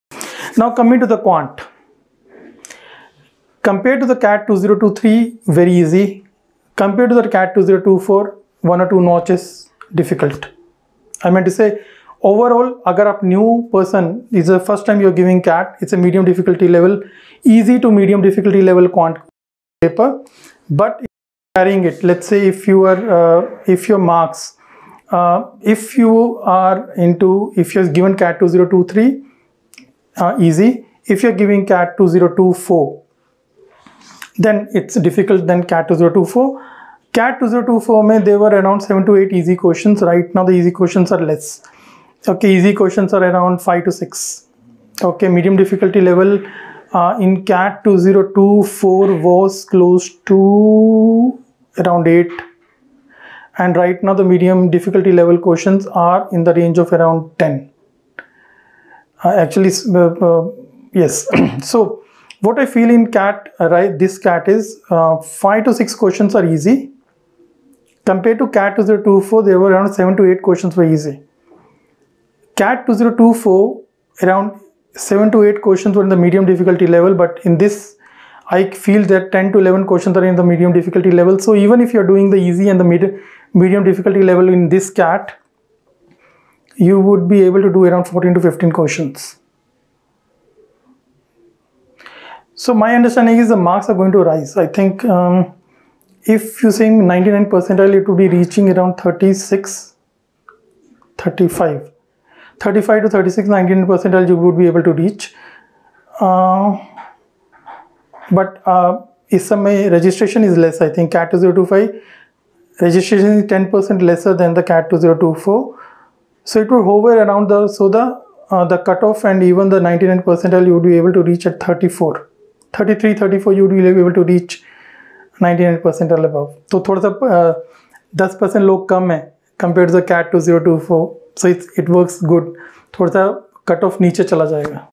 Was CAT 2025 Quant Tough? Quick Analysis by 99.99%iler 🔥
Was the Quantitative Ability (QA) section in CAT 2025 a nightmare or a breeze? Get a quick, expert reaction on the difficulty level from a serial CAT topper (99.99%iler)!

👇 FOR THE FULL, DETAILED QA BREAKDOWN & STRATEGY: 👇

Watch our complete long-form "Sectional Deep-Dive" video for a question-by-question analysis (easy/med/hard) and safe score prediction for QA